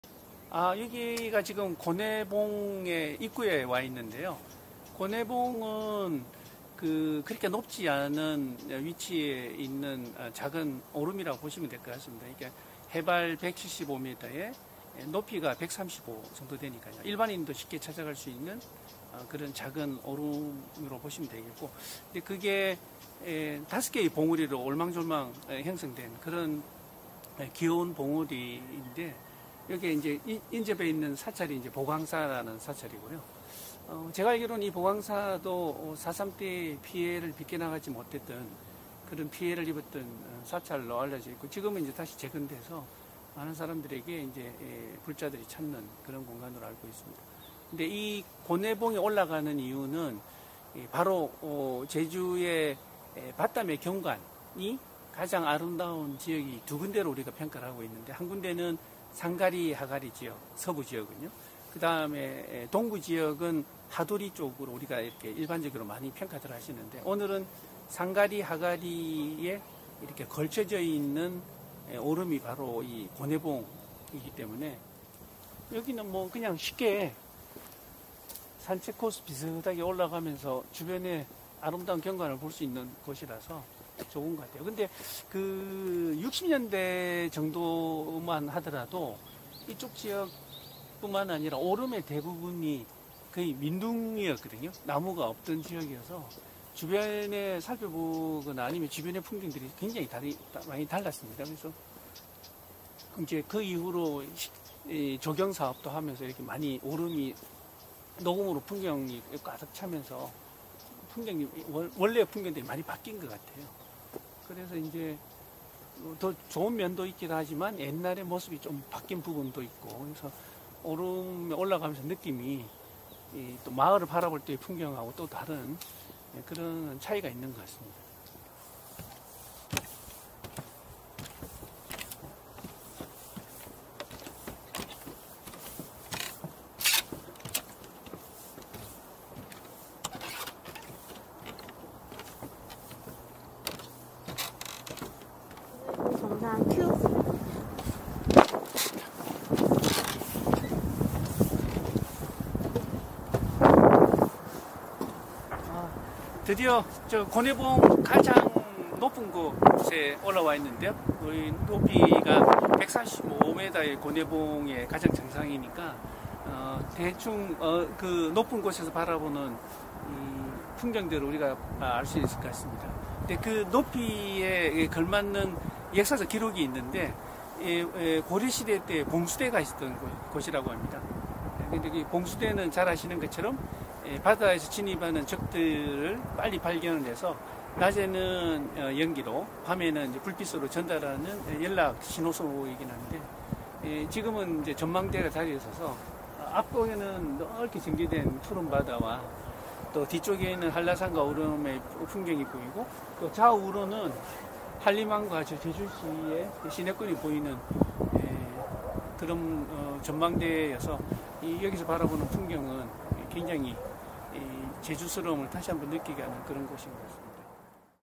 [360° VR 영상] 애월 고내리 원추형 오름 ‘고내봉’
JDC버스킹아카데미 랜선탐라기행
네번째 VR 영상  '고내봉' 영상 입니다.

[버스킹 명사]
제주대학교 건축학과 김태일 교수

클릭하여 체험해보시기 바랍니다.